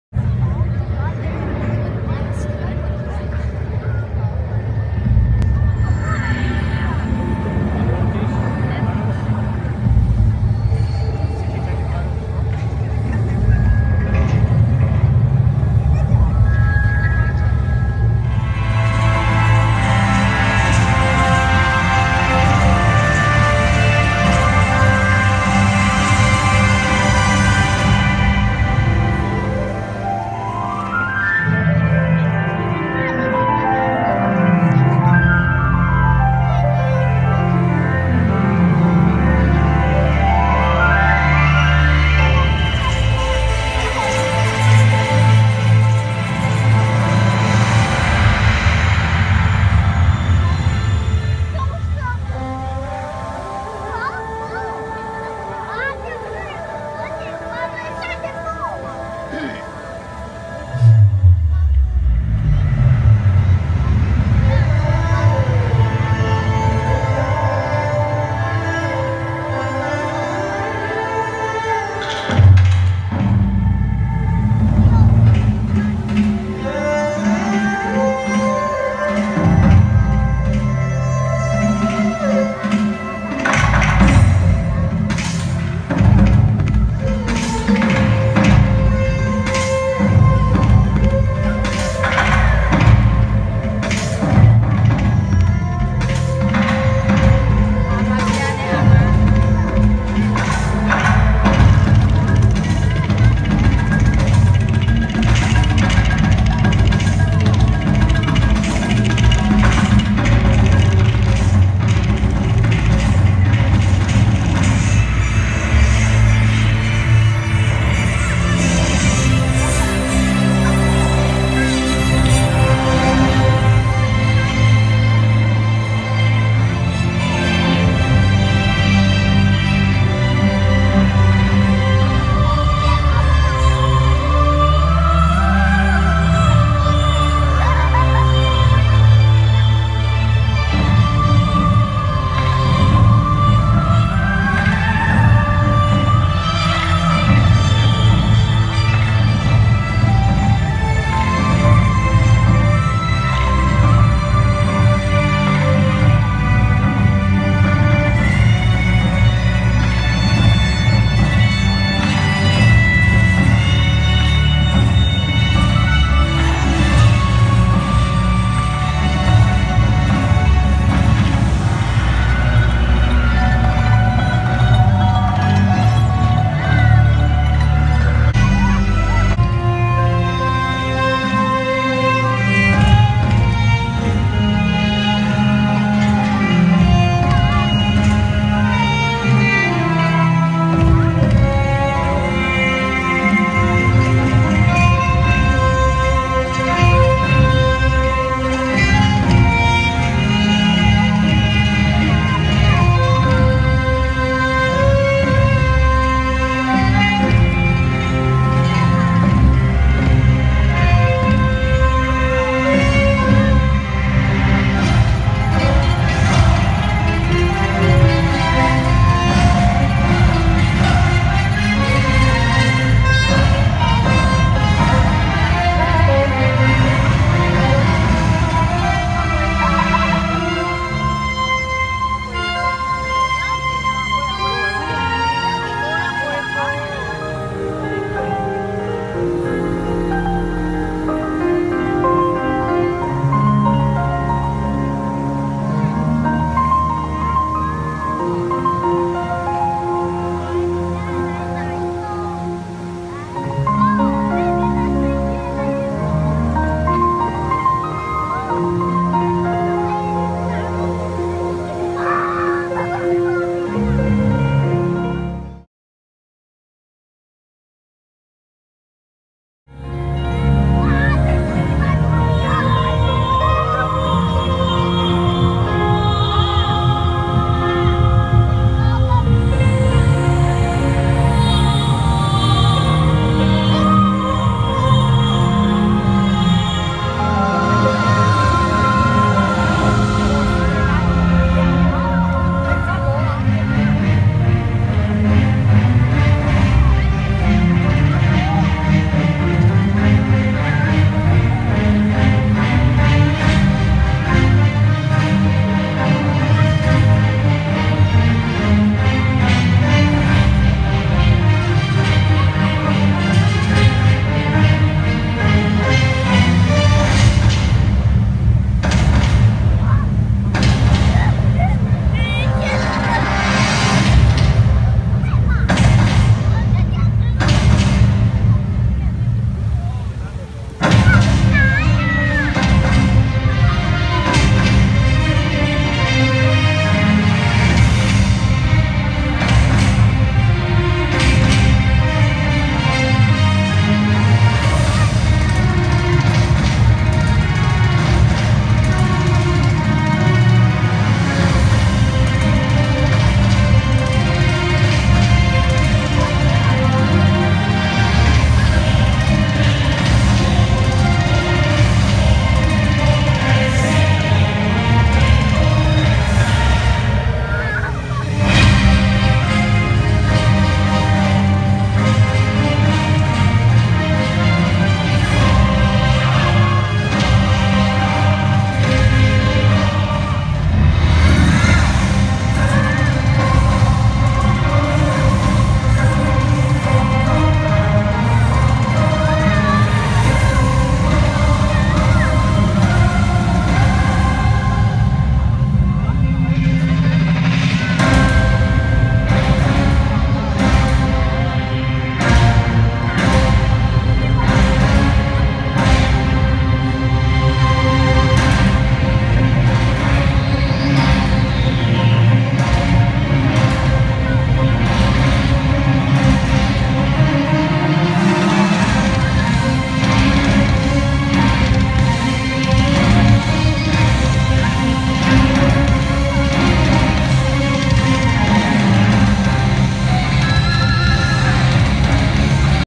MARINA BAY SANDS SINGAPORE ( SPECTRA - A light and water show )
Hello everyone. Let me introduce you the marina bay sands light and water show. Enjoy 🤗🤗